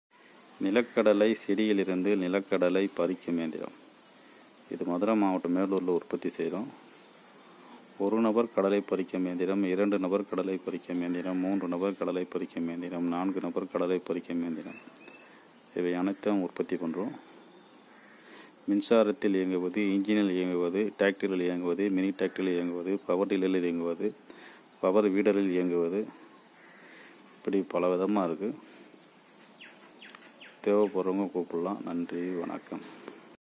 நிலக்கடலை பரிக்கும் இயத்திரம் விலை 6000 9787672239 மேலூர் மதுரை மாவட்டம்
Groundnut harvesting machine one man working machine RS 6000 or 9000,